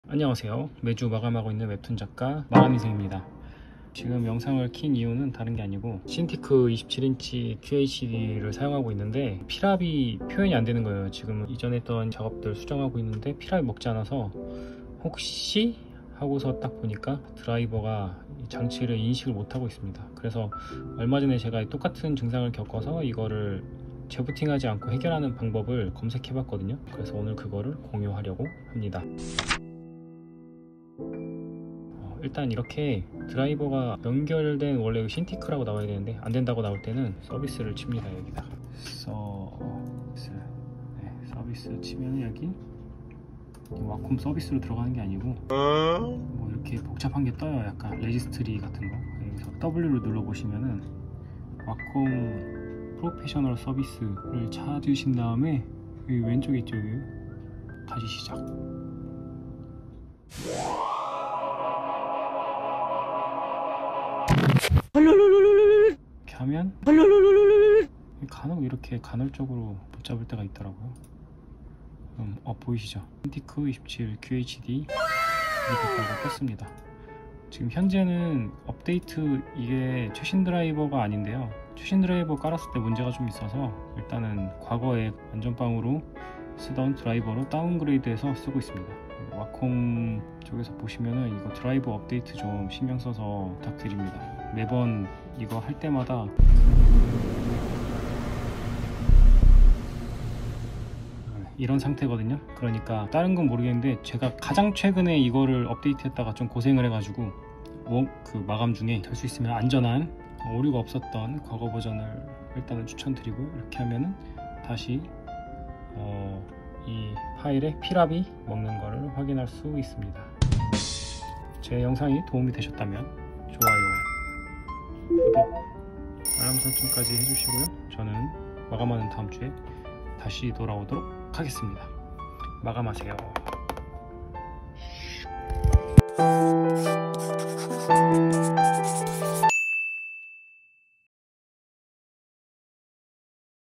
와콤 드라이버 인식 안될 때 재부팅 없이 해결하는 방법✍️
옛날엔 저도 이런경우 재부팅만 했었는데 꿀팁이 되시길!

자막 : vrew